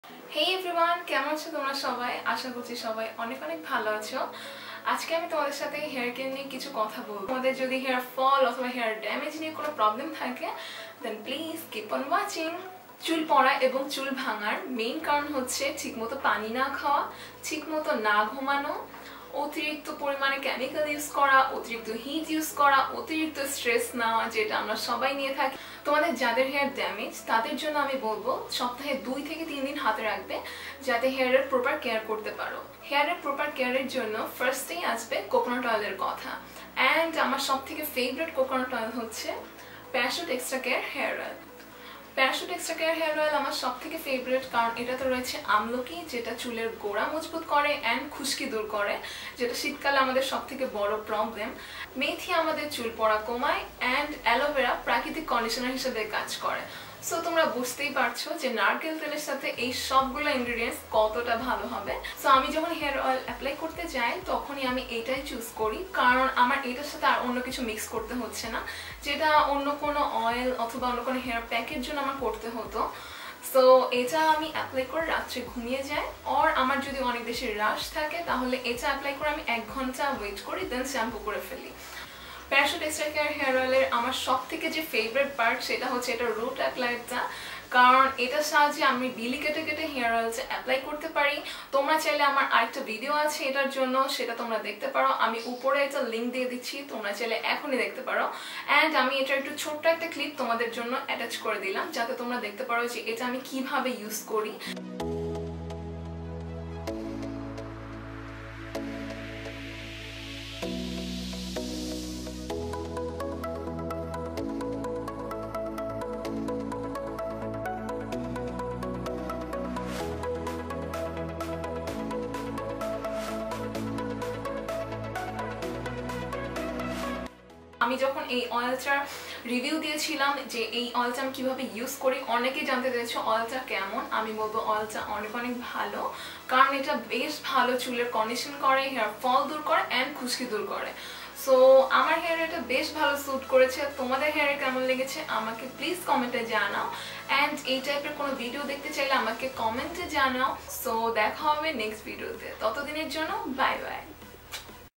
1 Simple way to Reduce HAIR-FALL | কিভাবে চুল লম্বা করবো? | Parachute Extra Care Hair Oil Review
Hey everyone
In this video I am talking about the Parachute Advansed Extra Care  Hair Oil which is prefect for hairfall or dameged hair.
this hair oil is enriched with Coconut Oil,amlaki, methi and aloe vera. It is good for all type of hair. Especially for those who has colored hair, and for those who regularly use many hair products for hair styling, applies heat. it also reduces hair breakage.